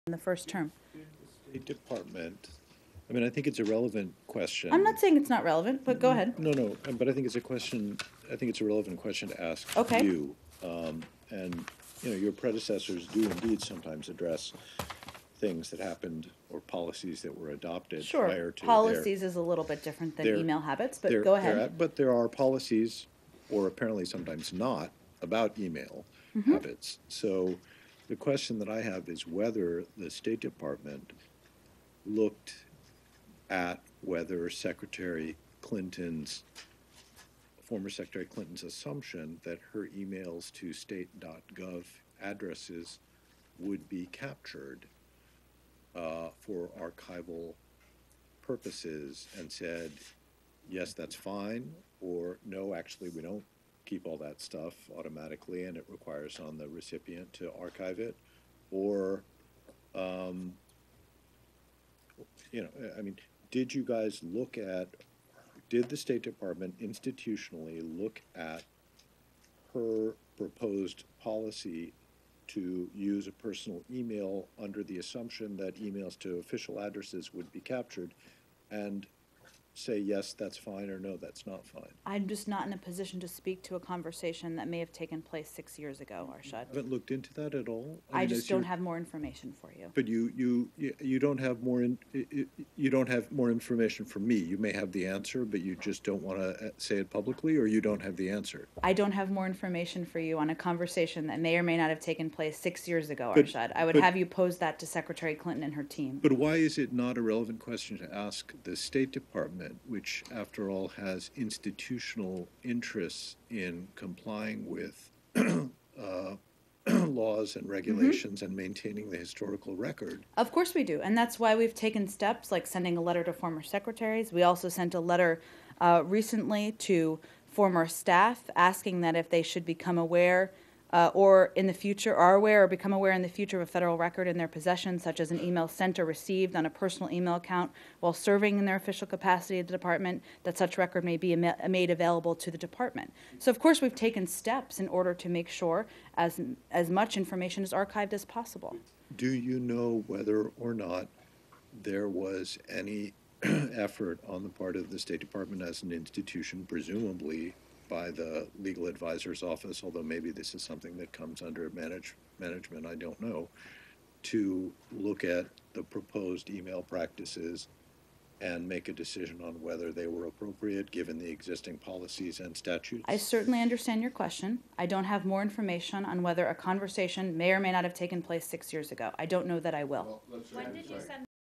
Psaki Won’t Say If State Dept. Looked At Clinton’s Email Policy
Psaki  Won’t Say If State Dept. Looked At Clinton’s Email Policy (March 13, 2015)